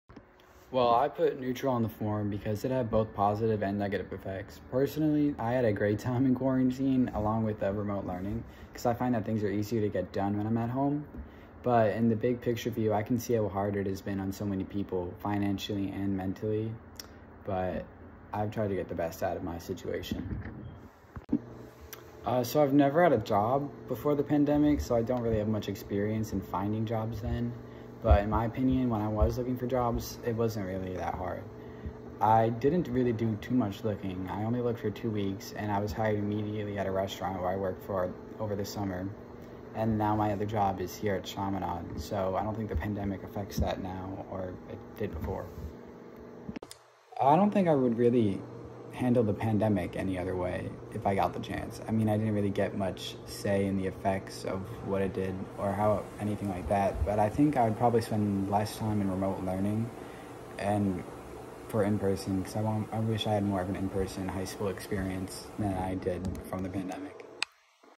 Ryan VerEecke Interview
1. On the form, you’ve stated that you felt “neutral” about COVID’s affect on your life. Can you explain your thoughts?
2. So I’m aware that you were looking for job opportunities during the pandemic. How affect your working experience and or limit the amount of jobs that were open for you
3. If you had the chance, would tackle the pandemic differently?